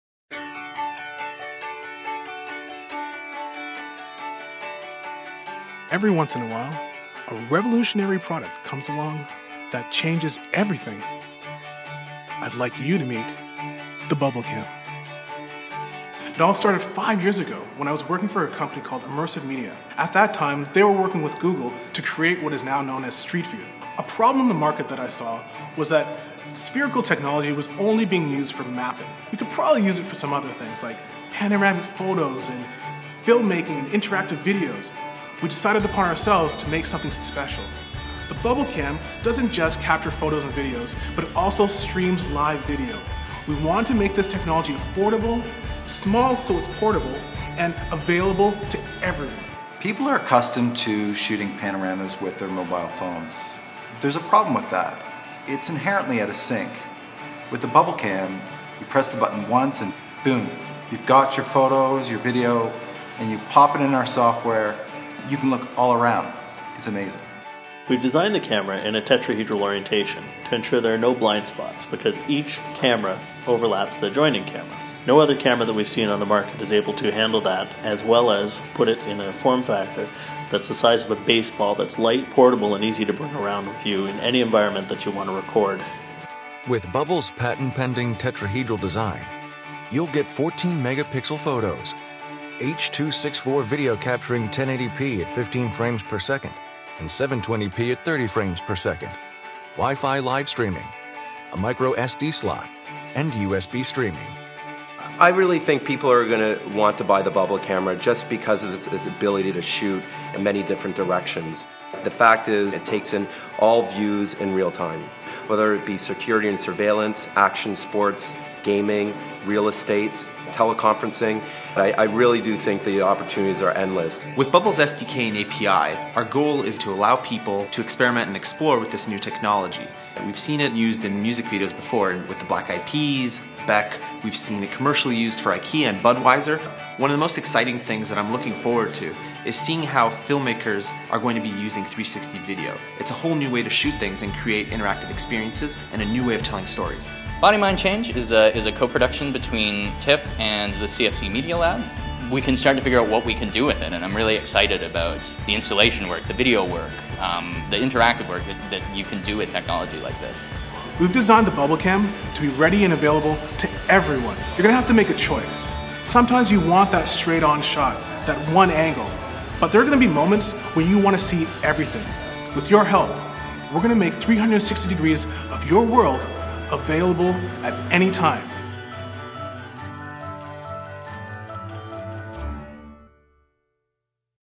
Bublcam 360º Camera Technology for Everyone
The bublcam is a 360º camera and software technology that shares everything around you through spherical photos and videos.